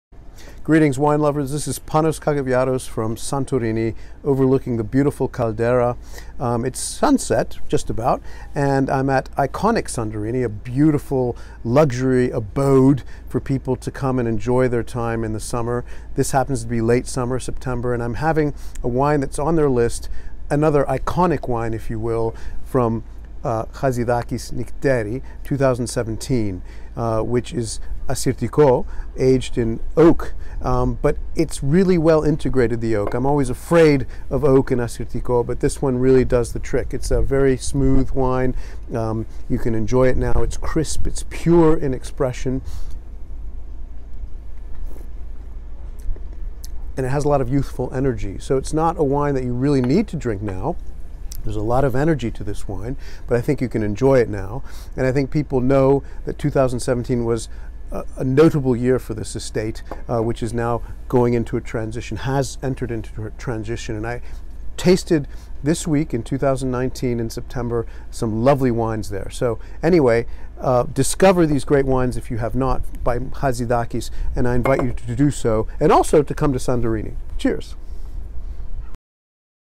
Hatzidakis Assyrtiko wine at Iconic Santorini
Just ahead of a great dinner at Iconic Santorini, we opened this lovely 2017 Nykteri from Hatzidakis, a very good vintage from oak aged Assyrtiko. The must was then fermented with indigenous yeasts at 18˚C. The wine was matured in oak barrels of different capacity for 12 months and then bottled unfiltered. Read more soon in wine-chronicles.com